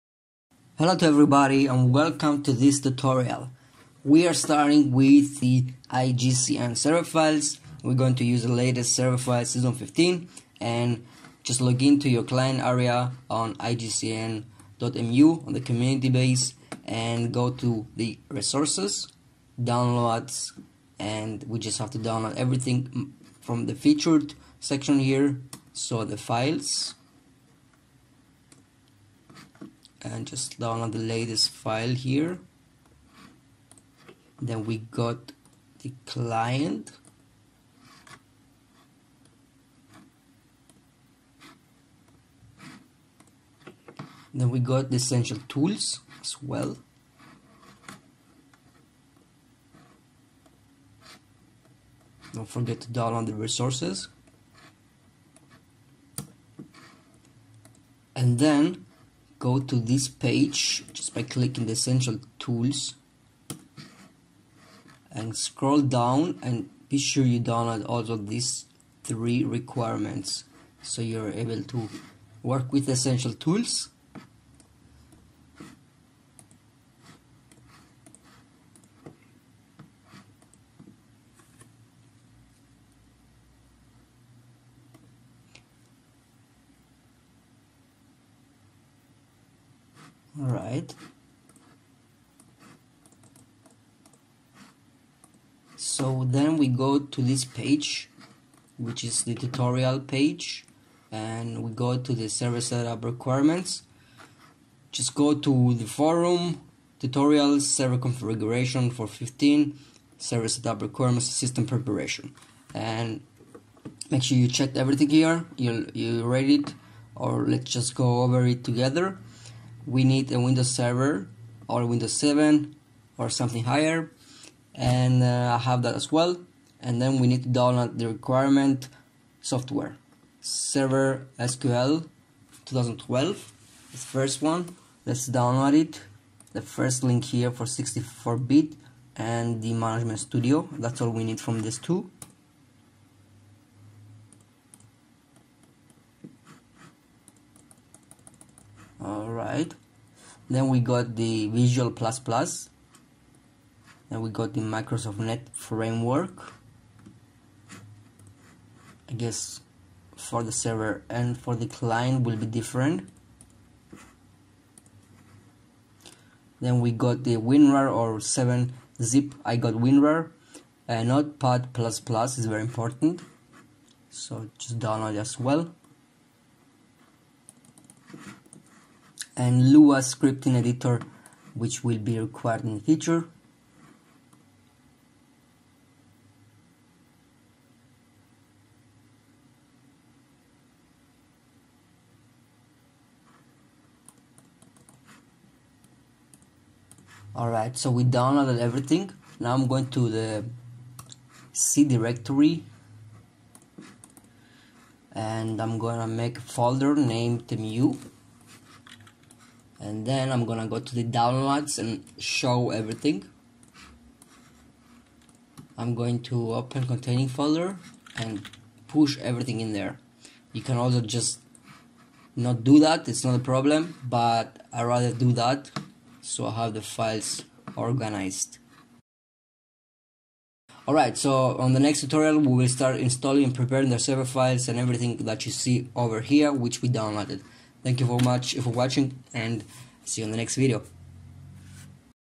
Setup [1] - Starting Up
A short presentation of getting started with IGCN Server files